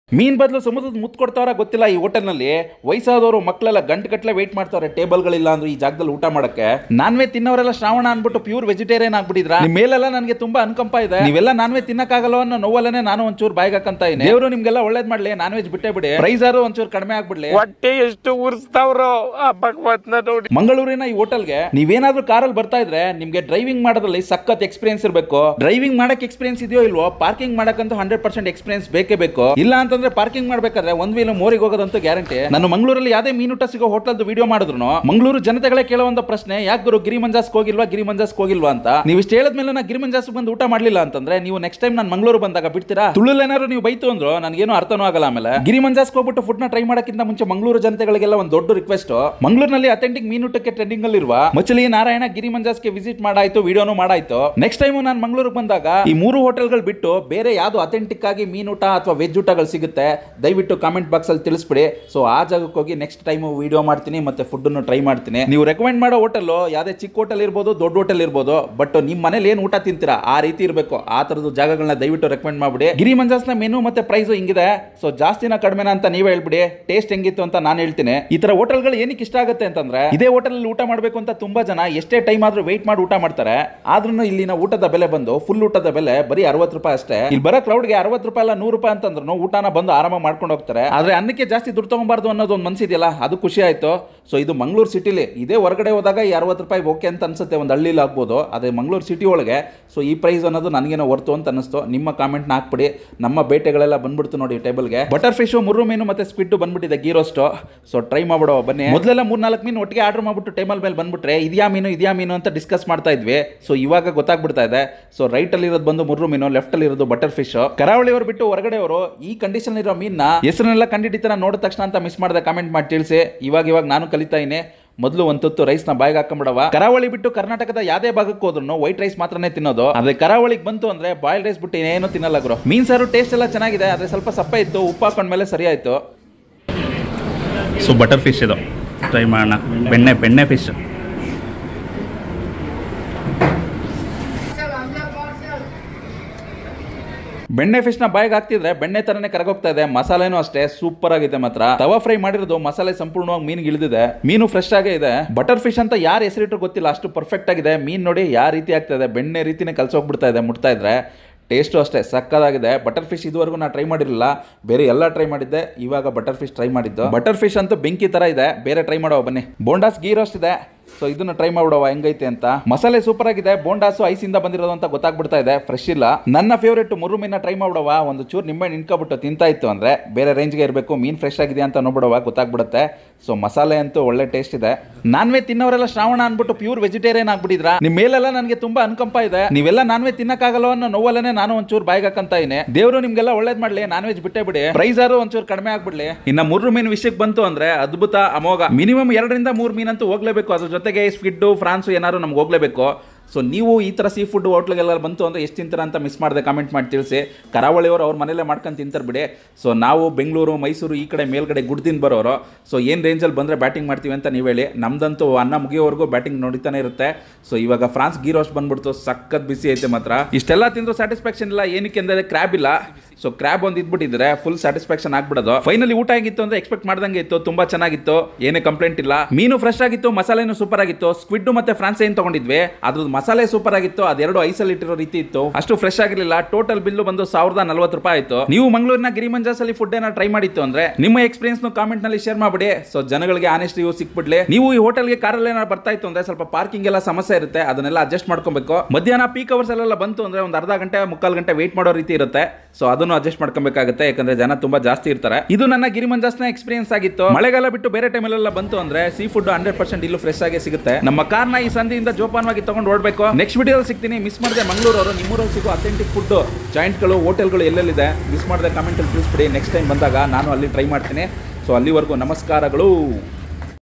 60rs Full ಮೀನು ಸಾರು ಊಟ | ಮಂಗಳೂರು ಮಹಾಜನತೆಗಳು ಹೇಳಿ ಮಾಡಿಸಿರುವ ವಿಡಿಯೋ ಇದು | Giri Manja's Hotel Mangalore
60rs Full ಮೀನು ಸಾರು ಊಟ | ಮಂಗಳೂರು ಮಹಾಜನತೆಗಳು ಹೇಳಿ ಮಾಡಿಸಿರುವ ವಿಡಿಯೋ ಇದು | Giri Manja's Hotel Mangalore